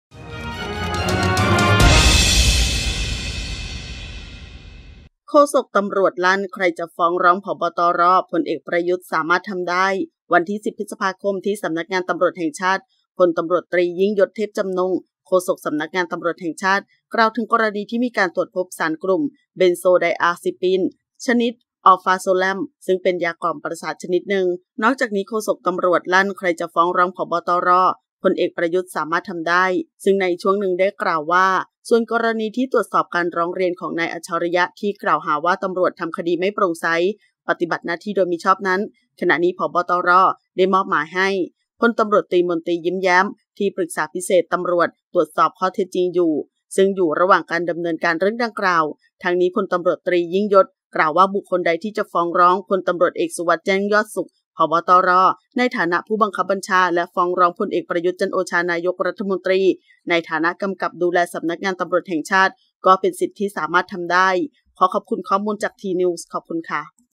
โฆษกตร. ลั่น ใครจะฟ้องร้อง ผบ.ตร.- พล.อ.ประยุทธ์ สามารถทำได้
วันที่ 10 พฤษภาคม 2565 ที่สำนักงานตำรวจแห่งชาติ (ตร.) พล.ต.ต.ยิ่งยศ เทพจำนงค์ โฆษกสำนักงานตำรวจแห่งชาติ ( โฆษก ตร.)​ กล่าวถึงกรณีที่มีการตรวจพบสารกลุ่มเบนโซไดอาซิปินส์ ชนิด Alprazolam ซึ่งเป็นยากล่อมประสาทชนิดหนึ่ง นอกจากนี้ "โฆษก ตร." ลั่น ใครจะฟ้องร้องผบ.ตร.- พล.อ.ประยุทธ์ สามารถทำได้

ส่วนกรณีที่ให้ตรวจสอบการร้องเรียนของ นายอัจฉริยะ ที่ กล่าวหาว่าตำรวจทำคดีไม่โปร่งใสปฎิบัติหน้าที่โดยมิชอบนั้น ขณะนี้ ผบ.ตร ได้มอบหมายให้ พล.ต.ต.มนตรี ยิ้มแย้ม ที่ปรึกษาพิเศษ ตร.ตรวจสอบข้อเท็จจริงซึ่งอยู่ระหว่างดำเนินการเรื่องดังกล่าว